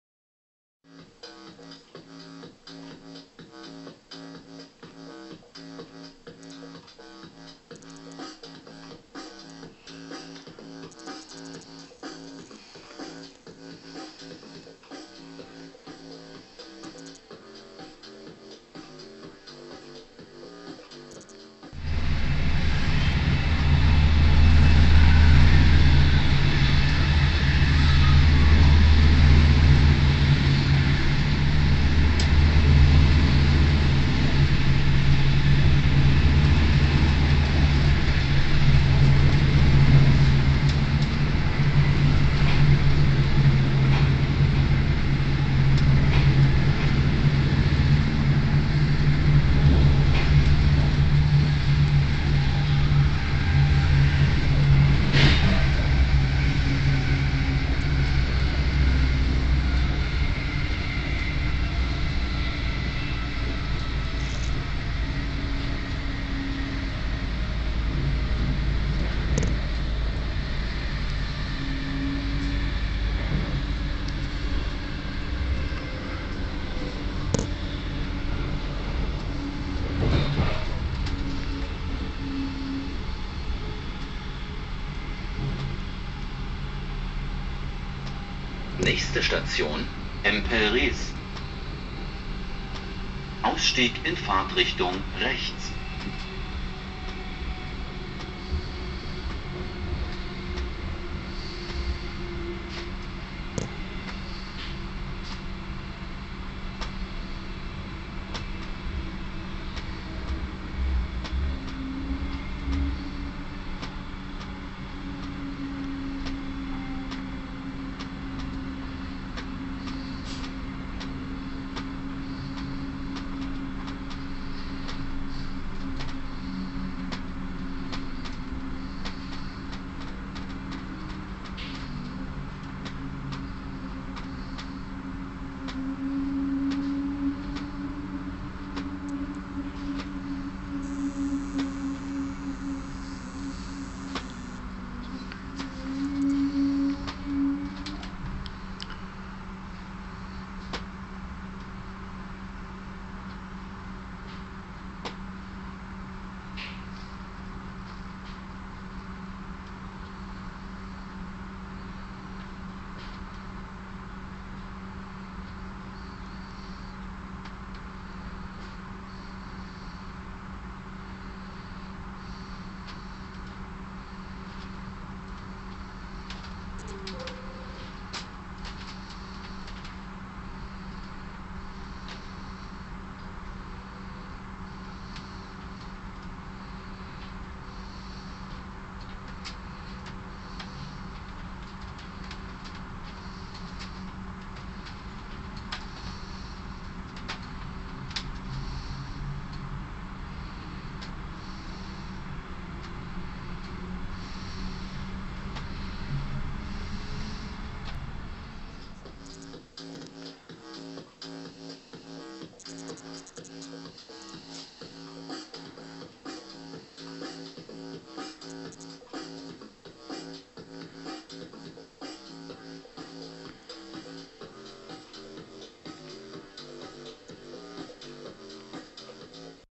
3 Minuten Alltag Empel Rees
Machen Sie eine kurze Pause, gönnen Sie sich drei Minuten Ruhe. Wir hasten durch den Alltag ohne wirklich zu sehen was um uns herum passiert. Sehen Sie Drei Minuten Alltag, machen Sie sich die Situation bewußt, genießen Sie die Ruhe im Bewußtsein eines Augenblicks.
Erleben Sie Drei Minuten Bahnfahrt zwischen Emmerich und Empel-Rees am Niederrhein.
Film: cc-by-sa-nc-nd. Mehr am Schluss des Filmes